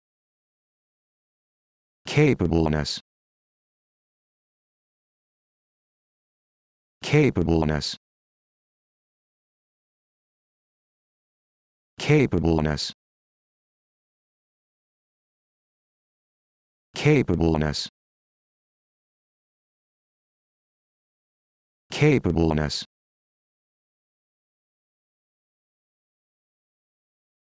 How to Pronounce Capableness
This video shows you how to pronounce Capableness